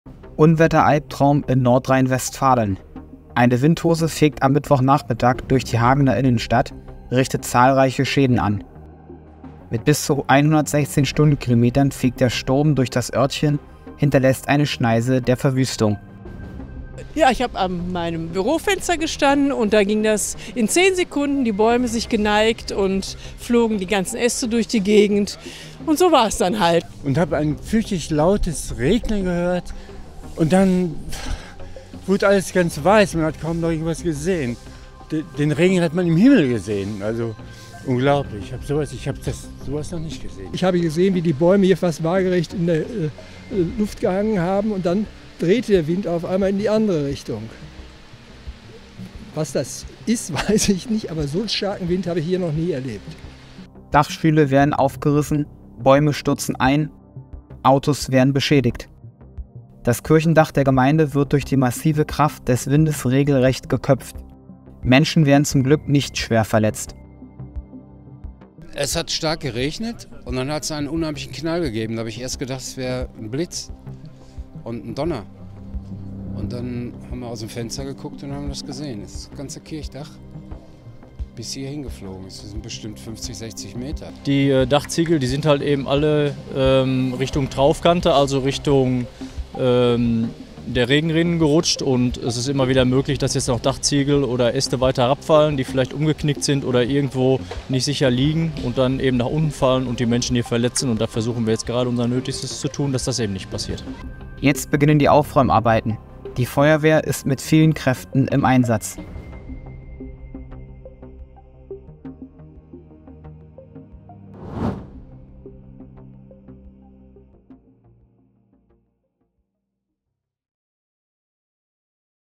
HAGEN, NRW: Windhose zerstört Innenstadt
NRW: Windhose zerstört Innenstadt
Am Mittwochnachmittag fegte eine Windhose mit bis zu 116 Stundenkilometern durch die Hagener Innenstadt und hinterließ eine Schneise der Verwüstung. Bäume neigten sich und Äste flogen durch die Gegend. Menschen wurden zum Glück nicht schwer verletzt, jedoch wurden Dächer aufgerissen, Bäume stürzten zu Boden und Autos wurden beschädigt. Jetzt beginnen die Aufräumarbeiten, wobei die Feuerwehr mit vielen Kräften im Einsatz ist.